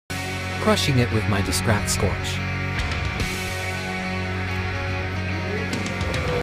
Scorching disc golf DRIVE 🔥🥏!disc
Started recently holding the disc further back in the reach back. Has helped me get a bit more slingshot motion on thr disc.

Still a work in progress but looking more promising. Here I am taking advantage of the change and crushing on my scortch.